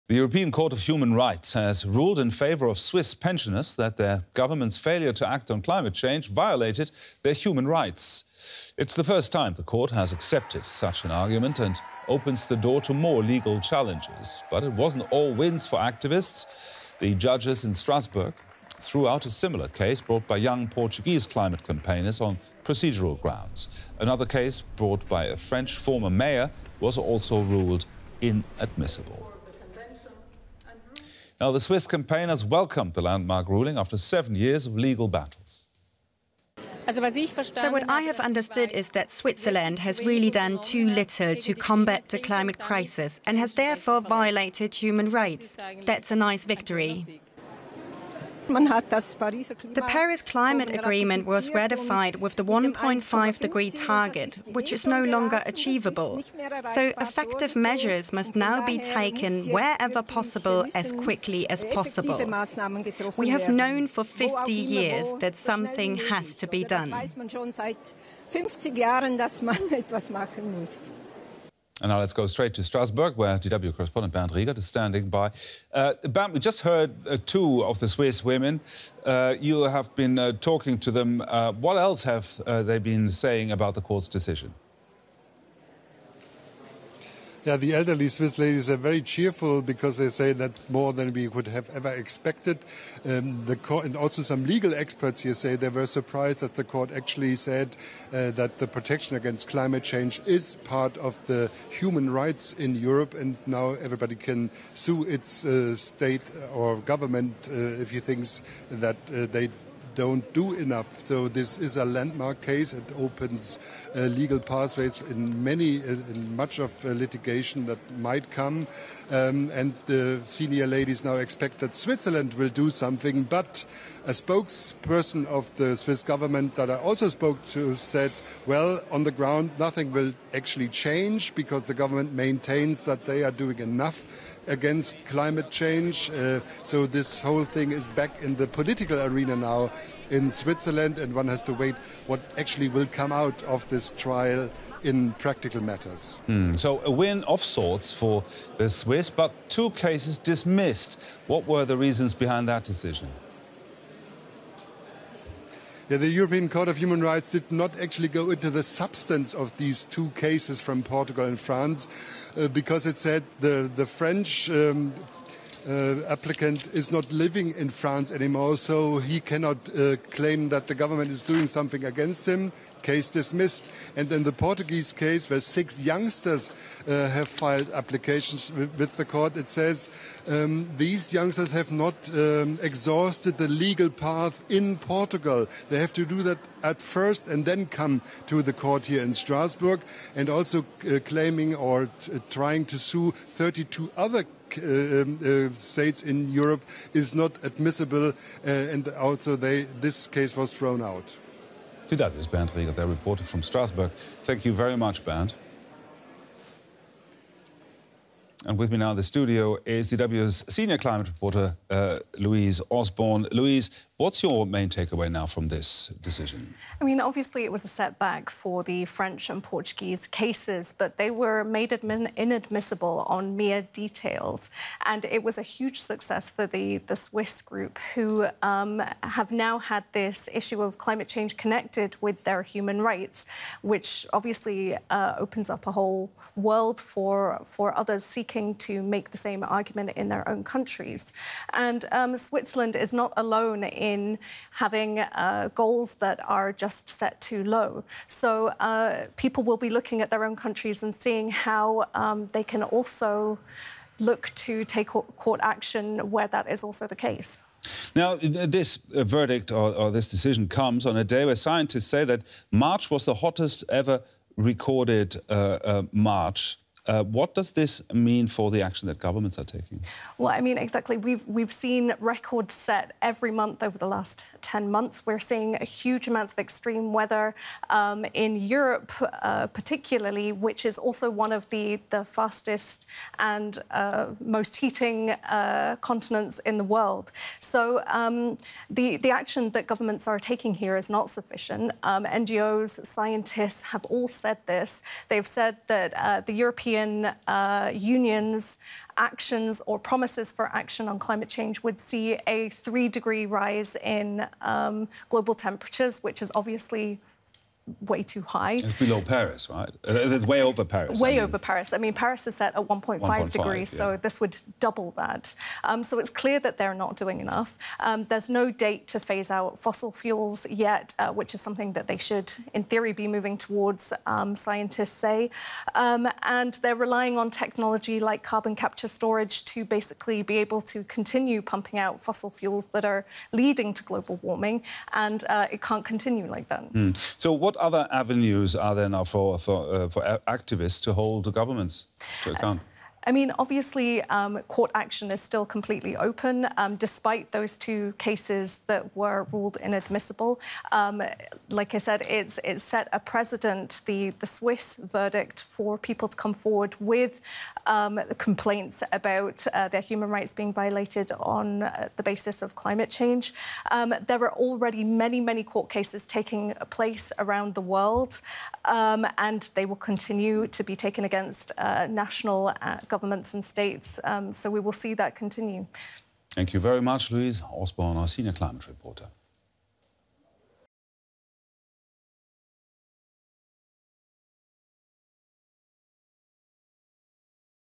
Swiss pensioners win climate victory in Strasbourg | DW News
The European Court of Human Rights has ruled in favour of Swiss pensioners that their government's failure to act on climate change violated their human rights.  It's the first time the court has accepted such an argument and opens the door to more legal challenges. But it wasn't all wins for activists -- the judges in Strasbourg threw out a similar case brought by young Portuguese climate campaigners on procedural grounds.  Another case brought by a French former mayor was also ruled inadmissible. We spoke to DW Correspondent Bernd Riegert in Strasbourg and to our Senior Climate Reporter Louise Osborne in Berlin.

00:00 Intro
01:11 Interview Bernd Riegert, DW Correspondent
03:27 Interview Louise Osborne, DW Senior Climate Reporter